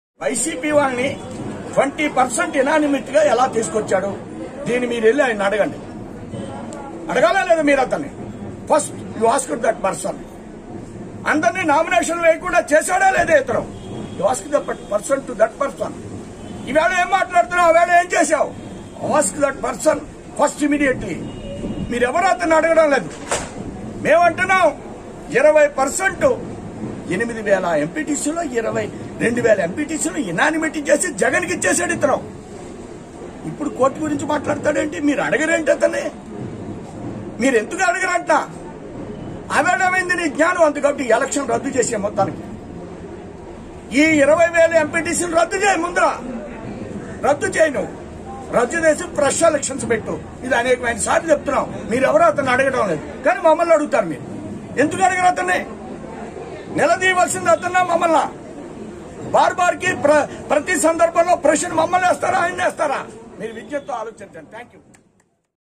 Bjp Somu Fires On EC | 2వేల ఎంపిటిసిలు వైసీపీకి అప్పనంగా ఇచ్చింది అతడే. రద్దు చేసి ఎన్నికలు పెట్టాలి
Bjp Ap President Somu Fires On EC || వైసీపీకి ఏకగ్రీవమైన పదవుల్ని రద్దుచేసి  మళ్లీ ఎన్నికలు పెట్టాలి..
మీడియా... మమ్మల్ని కాదు వాళ్ళని ప్రశ్నించాలి... సోము వీర్రాజు